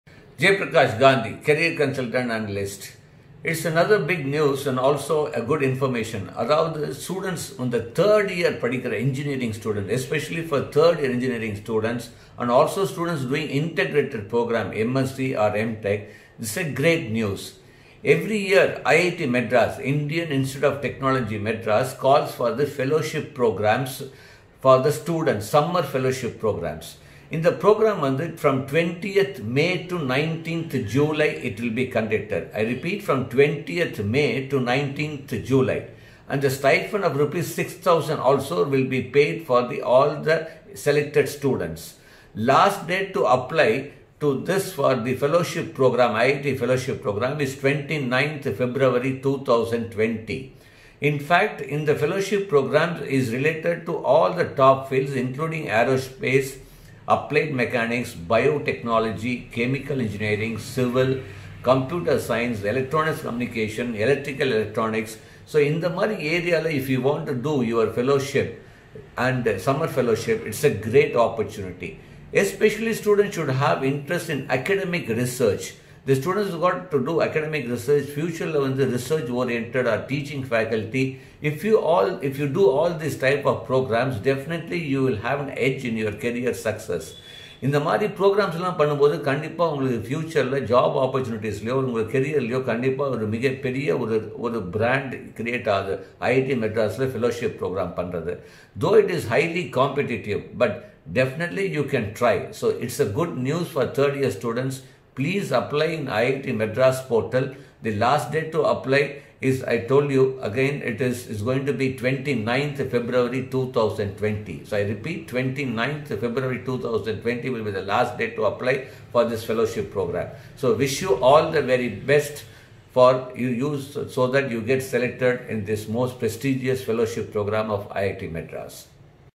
Insights about IIT Fellowship Program 2020 Opportunity for 3rd Year Engg Students
By Jayaprakash Gandhi - Career Guidance and analyst for over 19 years and counseled over 1.75 million students across the world.